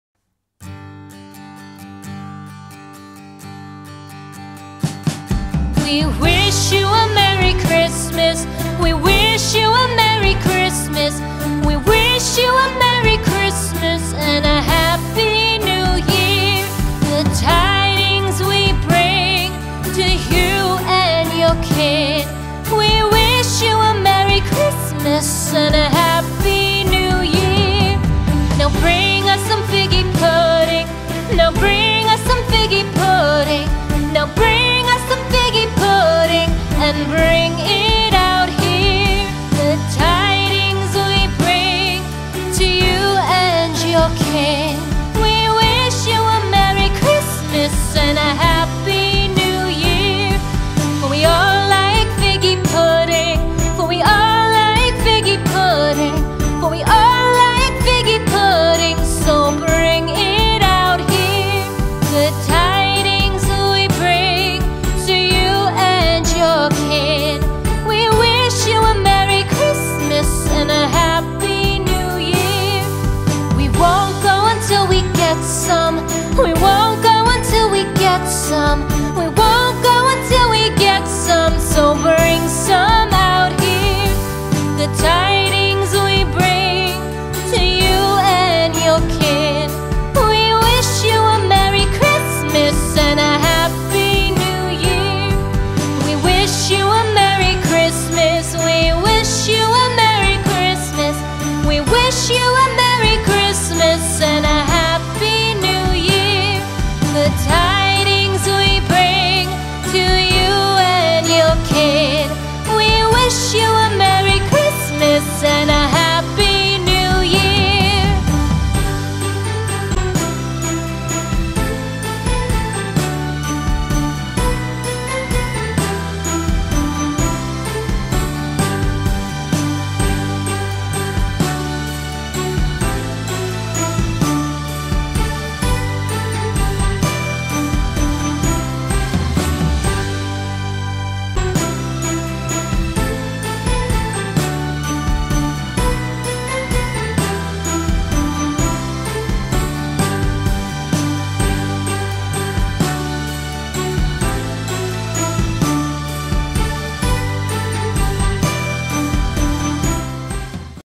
DIY | Christmas crib with snow effect.
Use your gift boxes to make Christmas crib @paper_rubies